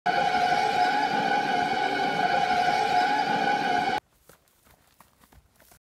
Pahadi_Bhoot(इस वीडियो को कमजोर दिलवाले ना देखें)
A rare footage of ghost in Himalayas, Warning - only for brave people.
The footage has been taken in Pauri Garhwal, Horrifying Footage that will blow your mind and Give you Goosebumps.